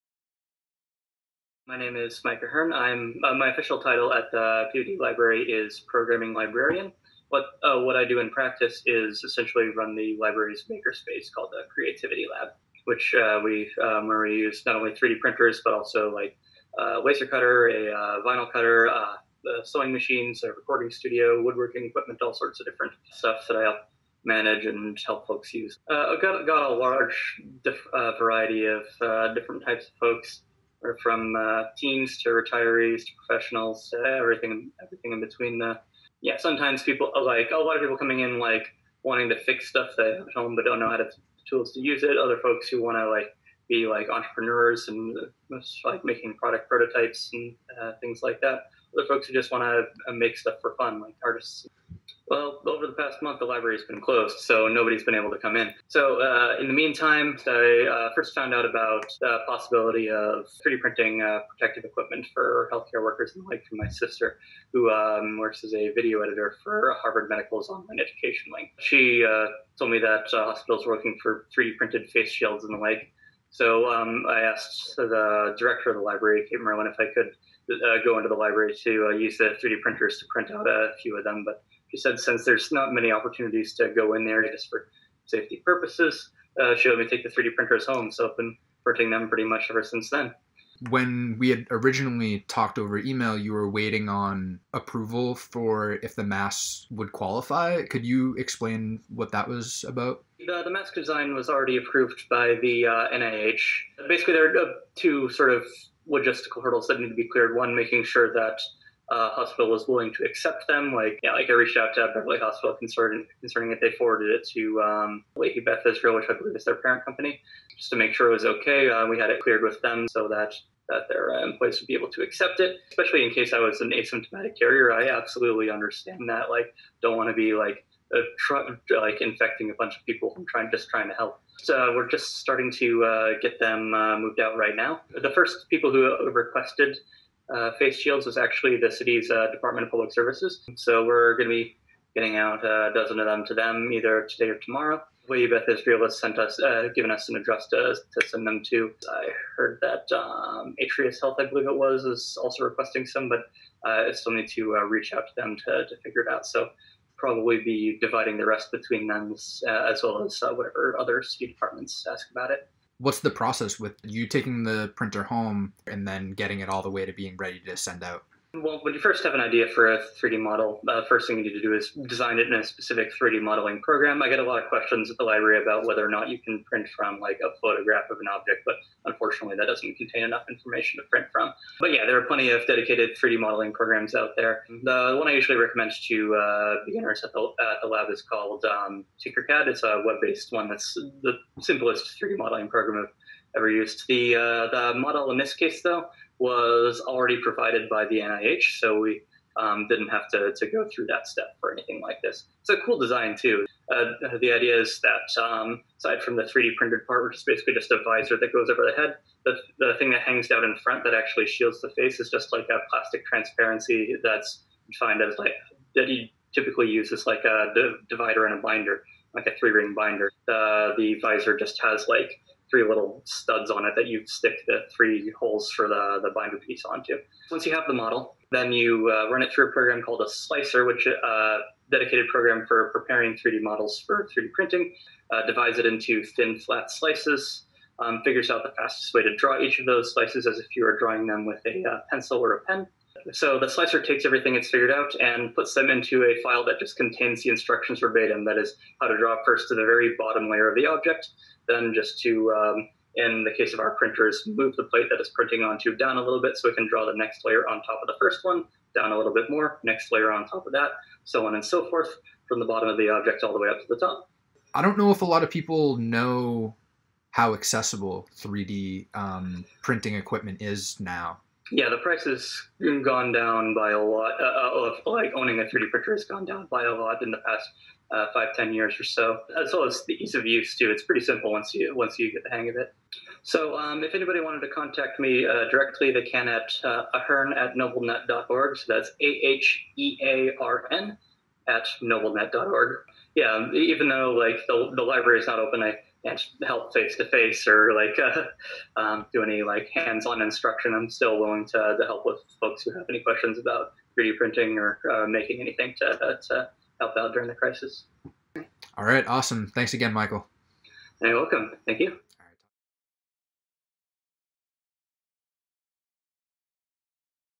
COVID Chronicles: Michael Ahearn
In Episode 10 of COVID Chronicles, Josiah sat down with Michael Ahearn from the Peabody Library. Michael has been 3D printing masks to assist our Department of Public Services.